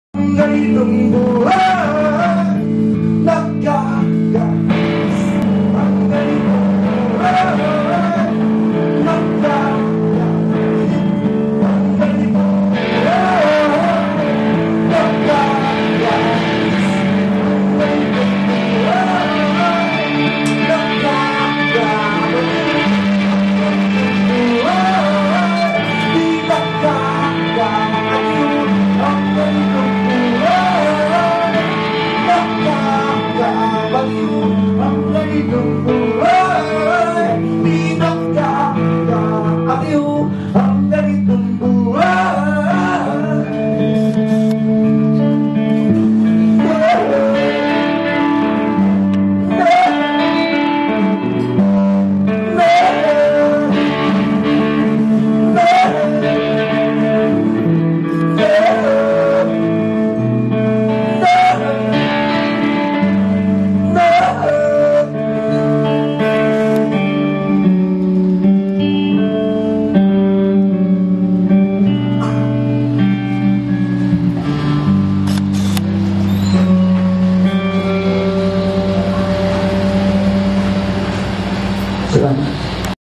Rock Ed Phils. x Dong Abay x ESEM x Kakoy Legaspi x Route 196 x for CDO/ILIGAN Relief
Canon EOS60D x 50mm Lens.